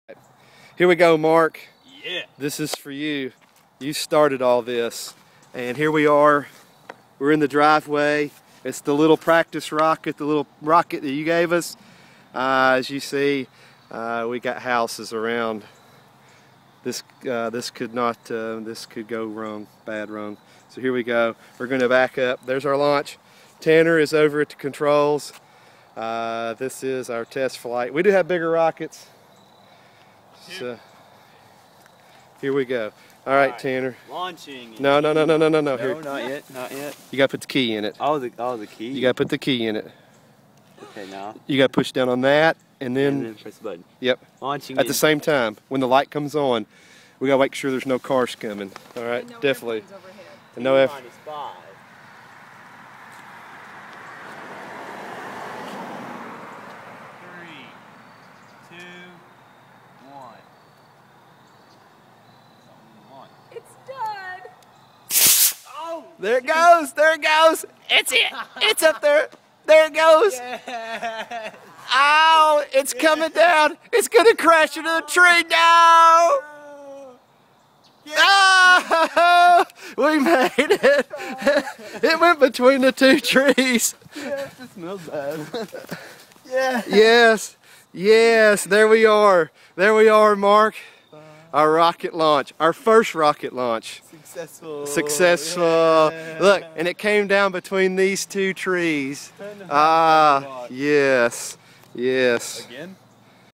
first launch
tanner and i launching our first model rocket. a gift from our friend Mark Bradley from Steeleville IL.